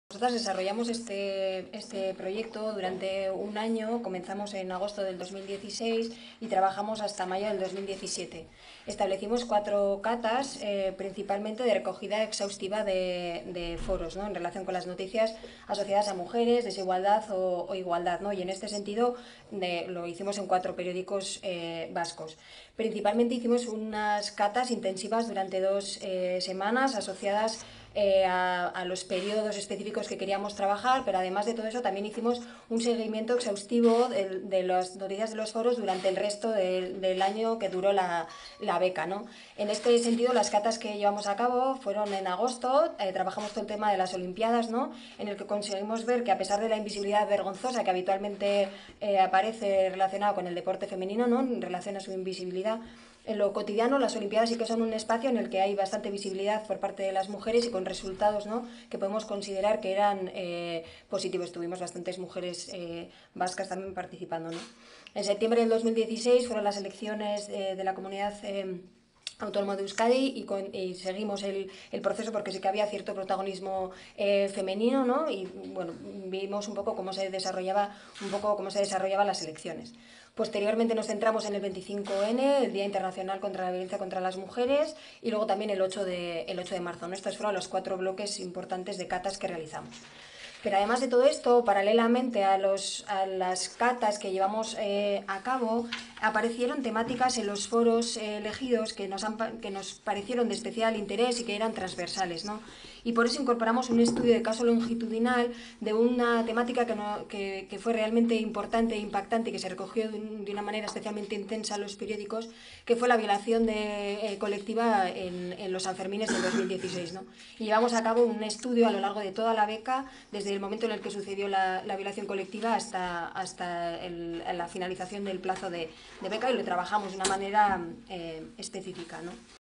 Análisis de los foros mediáticos: Metodología de trabajo (Rut Iturbide Rodrigo)
Un estudio becado por Emakunde destaca el alto grado de machismo que destilan los comentarios anónimos de algunos foros de participación de la prensa digital

Este vídeo es un extracto de la rueda de prensa de presentación del informe.

La investigación “Neomachismo ante las noticias sobre mujeres. Análisis de la participación del público en los foros mediáticos de la CAE”,realizada por Rut Iturbide, Patricia Amigot y María Isabel Menéndez y becada por Emakunde, se basa en un análisis con perspectiva de género de la participación del público a través de foros de prensa digital de la Comunidad Autónoma de Euskadi. El estudio se ha articulado a través del análisis de los foros de participación del público de cuatro rotativos que disponen de edición digital –El Correo, Noticias de Álava, Gara Naiz y Berria–, mediante un trabajo de campo realizado entre agosto de 2016 y marzo de 2017.

En muchos de los casos se niega o se cuestiona la existencia de ámbitos o fenómenos de desigualdad específicos. Los fenómenos más cuestionados, sea global o parcialmente, son la violencia de género, la feminización de la pobreza y las discriminaciones laborales de las mujeres. Esto abarca expresiones contundentes de negación de tales fenómenos, por un lado, y, sobre todo en el caso de la violencia de género, aparece un cuestionamiento indirecto mediante la sustracción de credibilidad a las víctimas. Los testimonios de las mujeres aparecen en muchos casos como sospechosos,  sin veracidad o como una invención interesada y terriblemente lesiva para los hombres. Uno de los ejemplos estudiados es el de la violación múltiple de Sanfermines, y en este sentido, se  detecta un importante volumen de intervenciones que implican sospecha y descrédito radical de la víctima.

Asimismo, aunque aparecen expresiones explícitas de sexismo y denigrantes o de odio, tienen una mayor presencia las expresiones más sutiles de falta de reconocimiento de las mujeres –de su credibilidad, de su capacidad, de su éxito, etc.–. El no reconocimiento de los saberes de las mujeres viene acompañado en muchos casos de actitudes aleccionadoras (mansplaining).

Asimismo, se detectan intervenciones que sugieren la existencia de trolls, por su aparición constante y persistente en relación a temáticas relacionadas o no con las noticias en las que aparecen, siempre destinadas a cuestionar la igualdad.

¡Te esperamos!